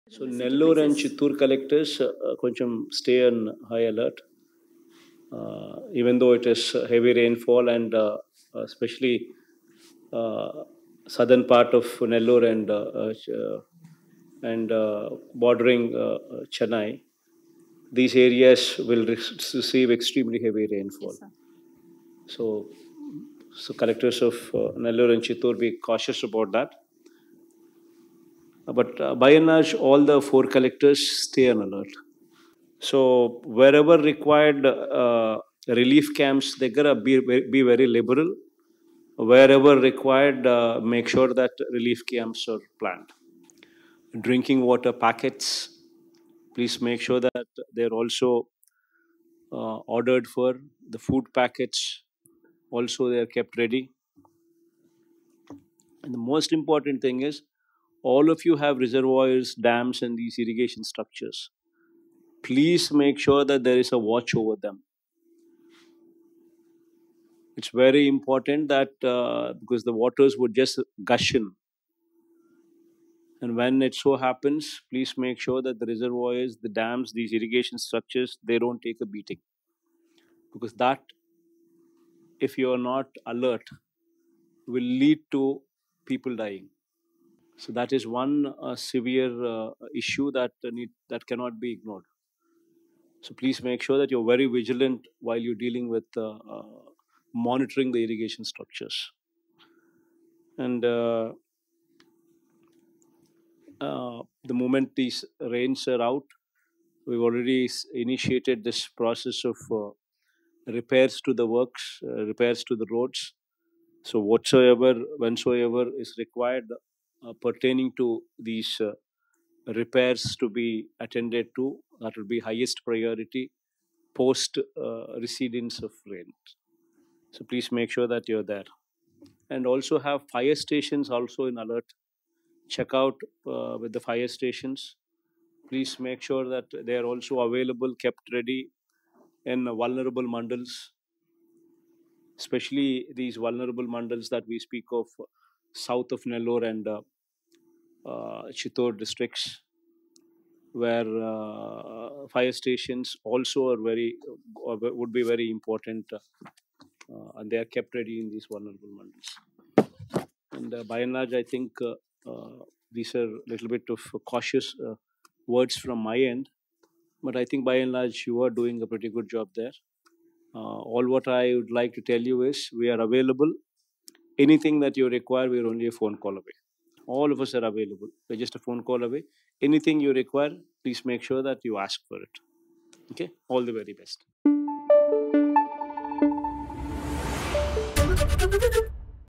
CM Jagan Review : రాయలసీమలో కురుస్తున్న భారీ వర్షాలపై సీఎం సమీక్షా సమావేశం | ABP Desam
రాయలసీమలో భారీ వర్షాలు కురుస్తున్నాయి. ప్రకాశం, నెల్లూరు, చిత్తూరు, కడప జిల్లాలో జోరుగా వర్షాలు కురుస్తుండటంతో లోతట్టు ప్రాంతాలన్నీ జలమయమయ్యాయి. క్యాంప్ కార్యాలయం నుంచి వీడియో కాన్ఫరెన్స్ నిర్వహించిన సీఎం. నాలుగు జిల్లా అధికారులను క్షేత్రస్థాయి పరిస్థితులు అడిగి తెలుసుకున్నారు. అనుక్షణం అప్రమత్తంగా ఉంటూ ప్రజలకు అందుబాటులో ఉండాలని ఆదేశించారు.